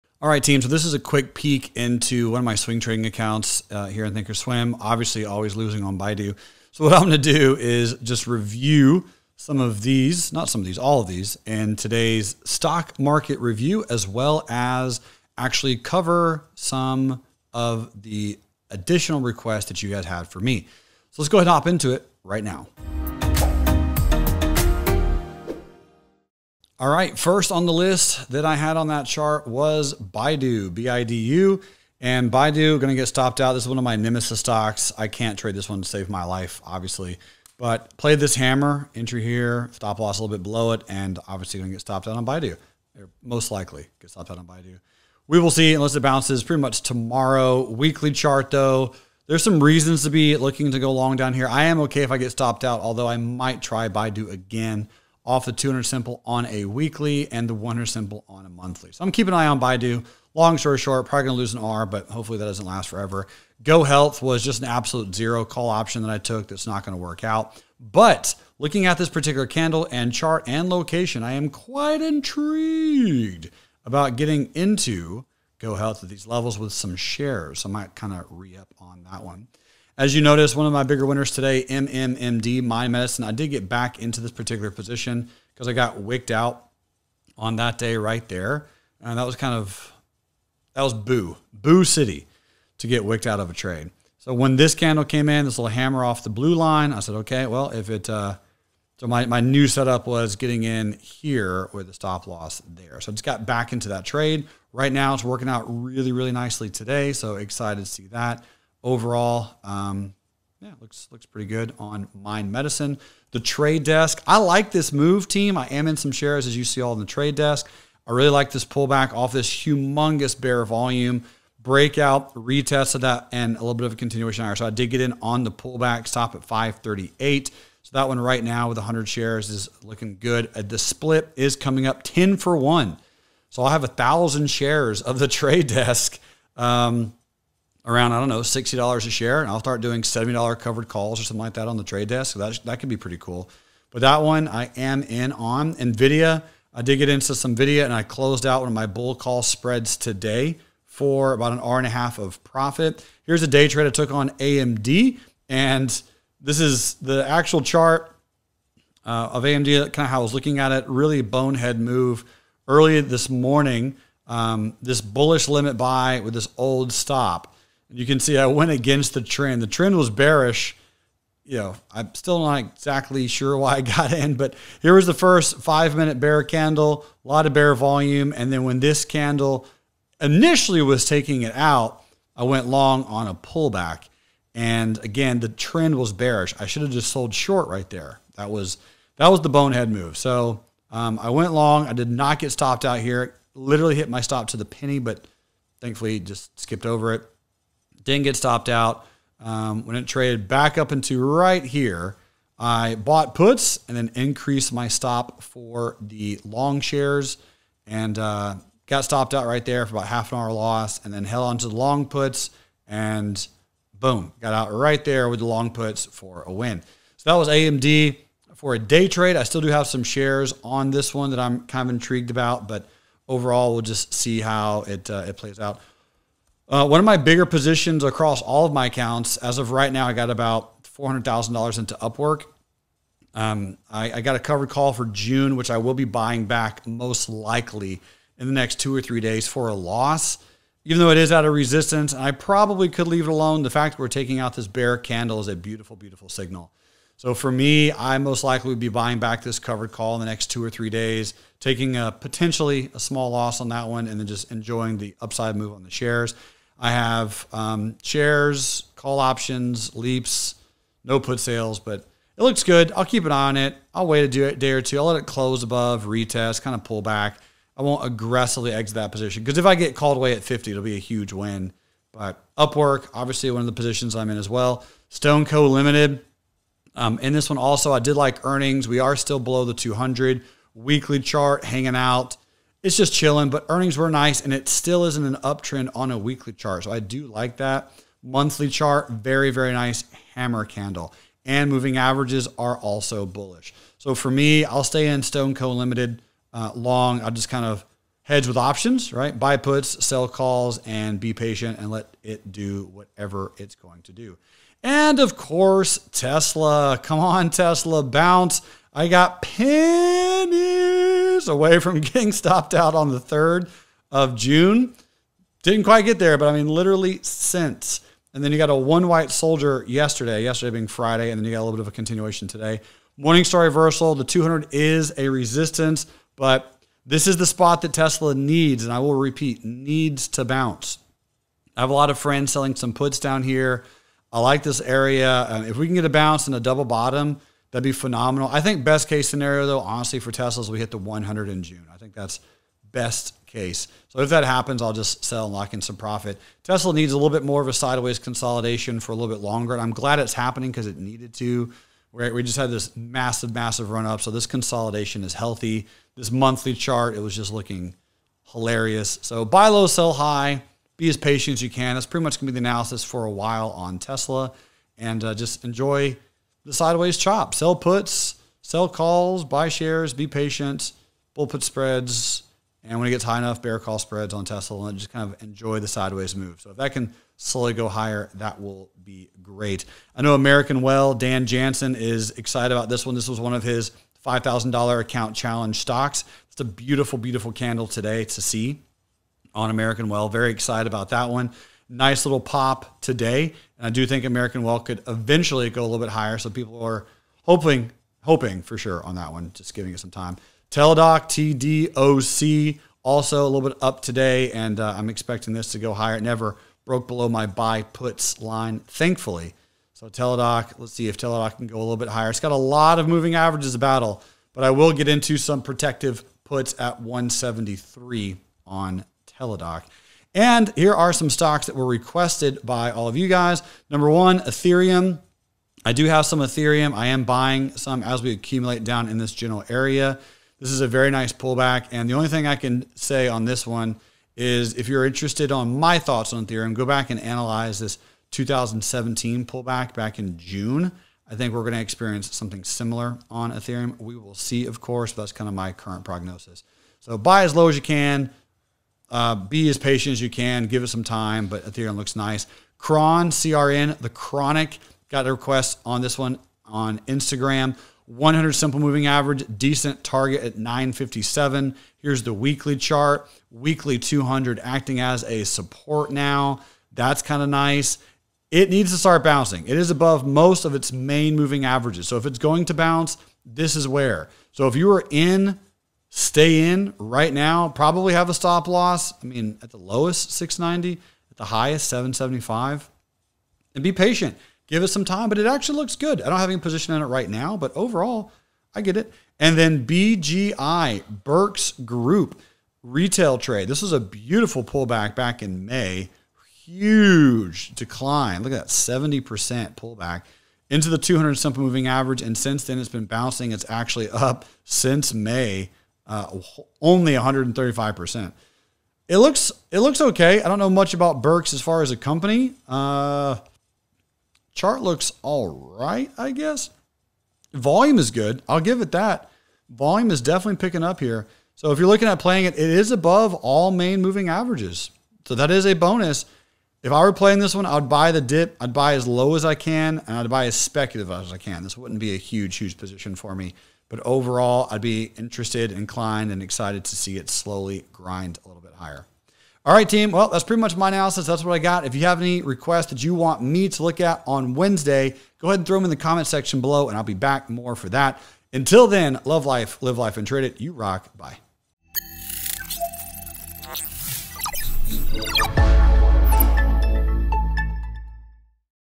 Peak Into One of Jerremy Newsome's Swing Trade Accounts
Take a peak into one of Jerremy's swing trade accounts. He talks about his current swing trades as well as breaks down some price action from today.

💸 Want a Funded Account? Learn how you can get 200k to Trade